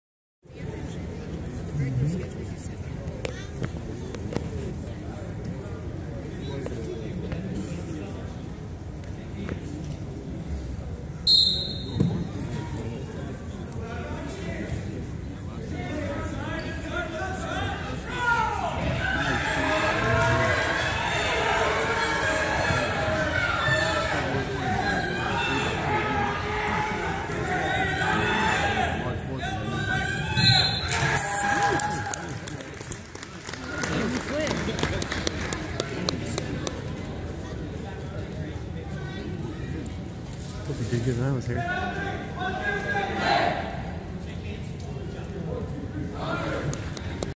RCMC Wrestling 2016
Rodney's match at West Central, his first win.